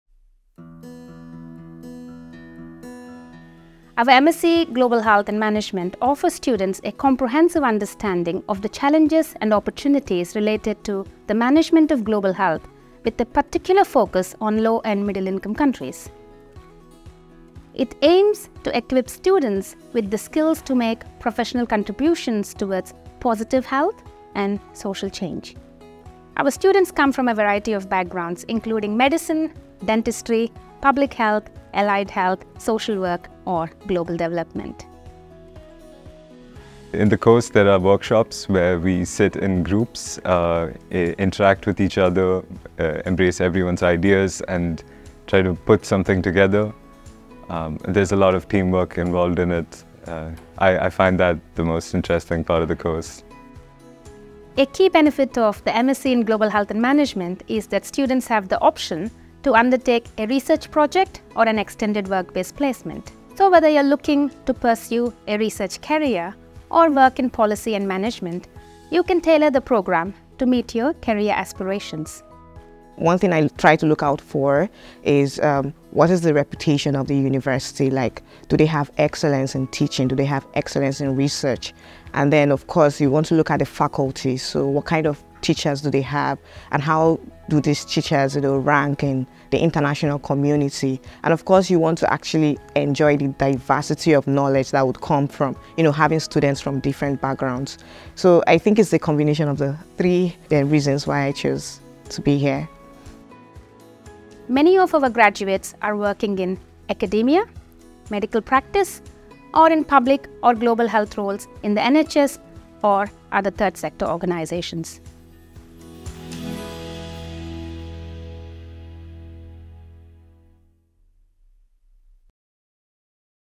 MSc Global Health and Management at Aberdeen
Our Masters in Global Health and Management is ideal if you are looking for a postgraduate degree that will equip you with knowledge and skills to address complex and multidimensional global health issues, enable you to make professional contributions towards positive health and social change, whilst focussing on your employability.

In this video, Dr Aravinda Guntupalli introduces you to the degree programme and your career prospects after graduating, and students talk about why they chose to study the programme at the University of Aberdeen.